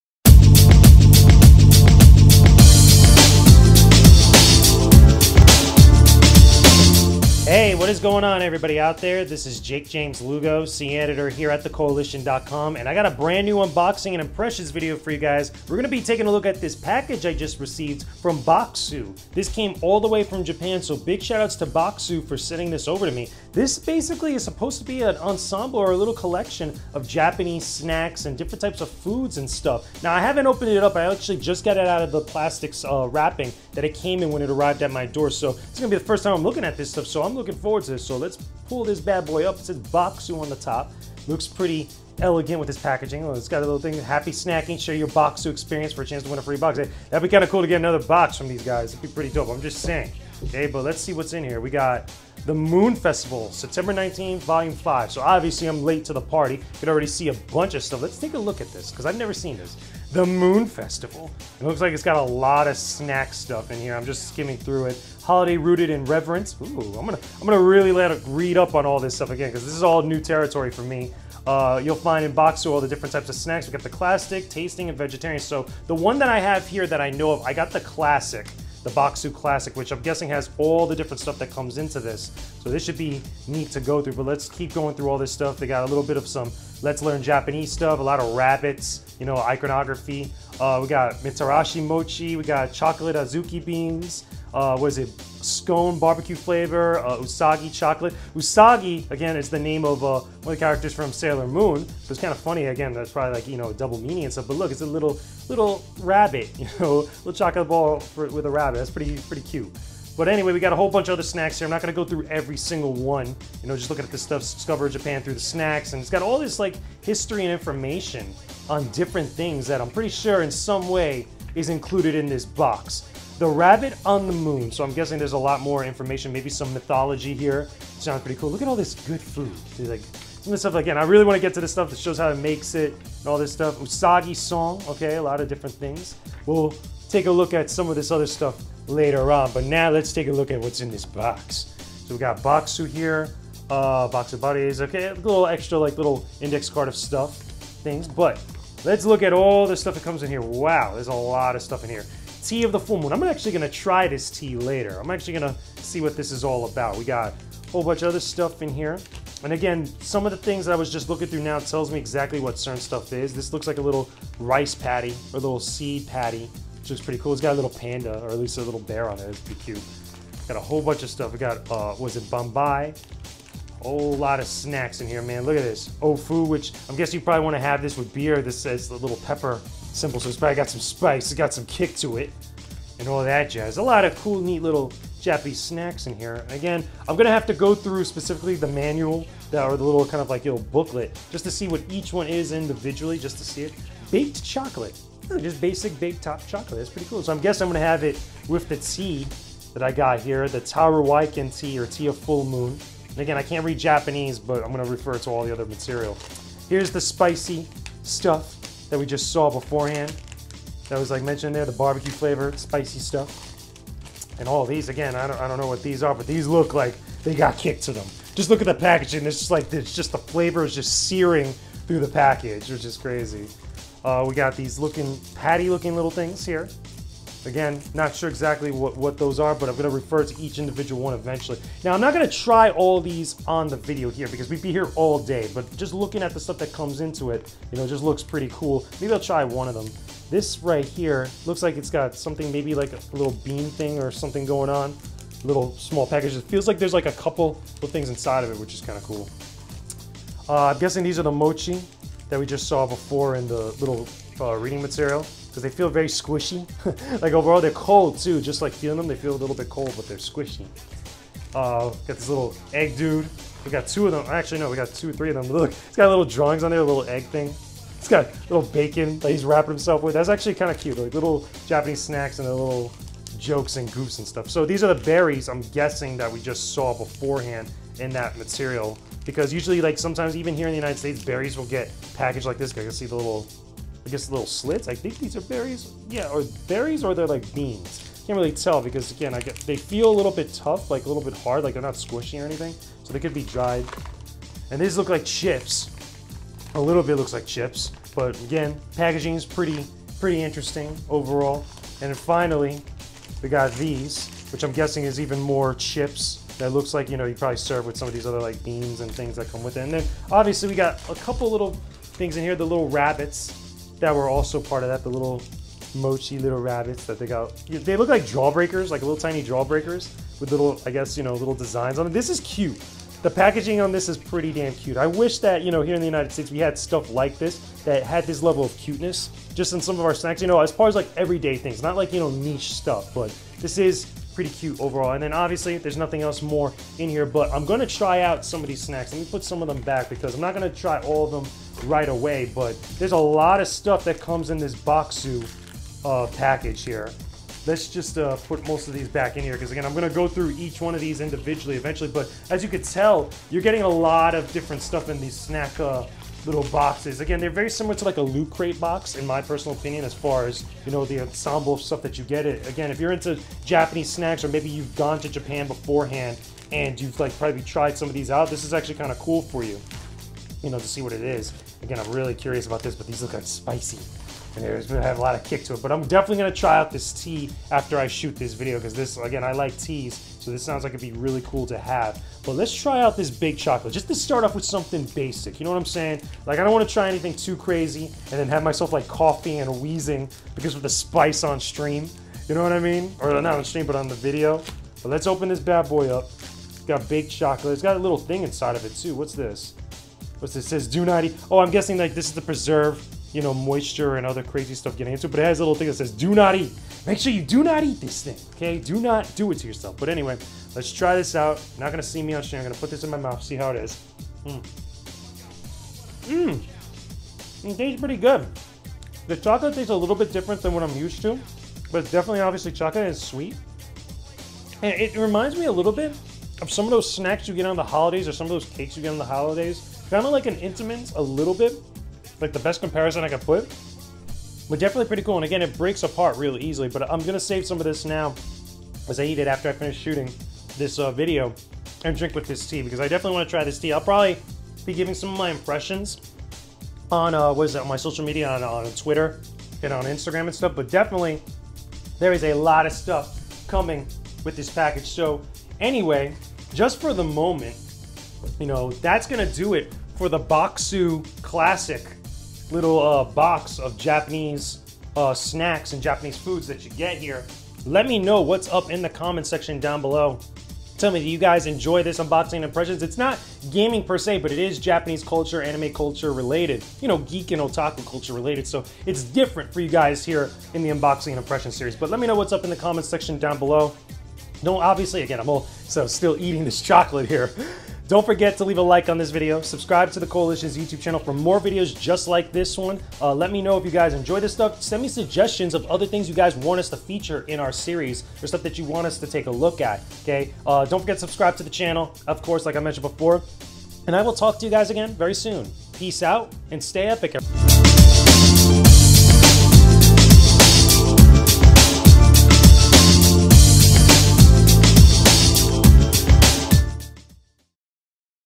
Bokksu Moon Festival Classic Unboxing and Impressions - The Koalition
In this unboxing and impressions video, Senior Editor Jakejames Lugo checks out the Bokksu classic box for September. This box is themed to the Moon Festival in Japan and comes with a selection of snacks and treats to celebrate the festival. Along with a detailed description of each snack, the box also includes some histroy and trivia about Japan and the Moon Festival. Should you dig into all these Japanese snacks? Or stay far away from this box? Watch the full unboxing and impressions to find out!

The Koalition provides videos and podcasts relating to the latest news in both gaming and pop culture.